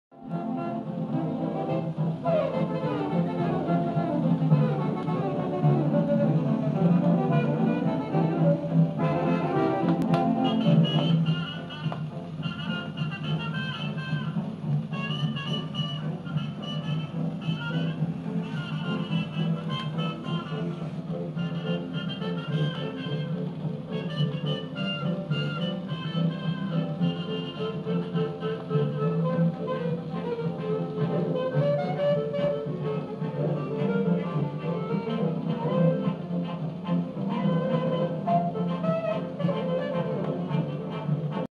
Paillard Radio Phono 1937 first song after more than 70 years idle.
first run but motor had to be cleaned and regreased and Pickup reproducer needed rubber dampers replacement